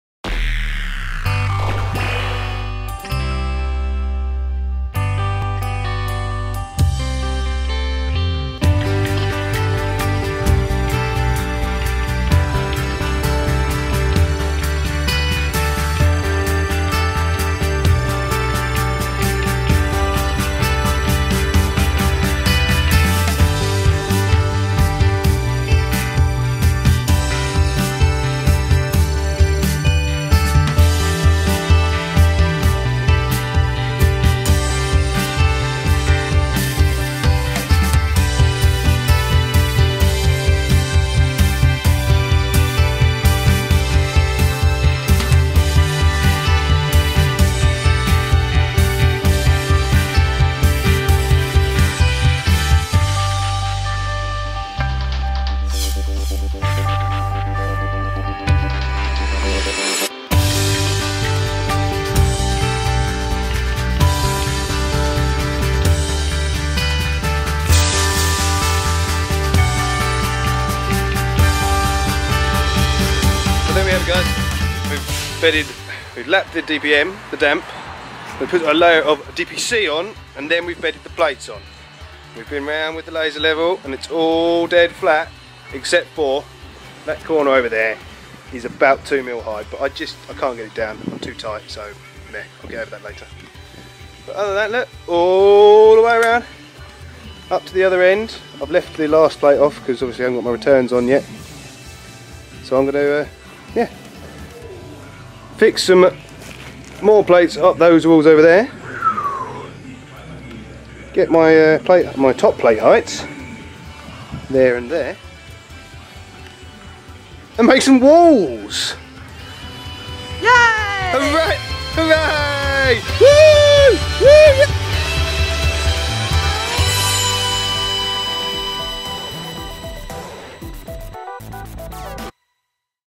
Bedding The Plates Part 2
Well it didn't rain at all :D
next job, strap them down and get making some walls. hurray

Ed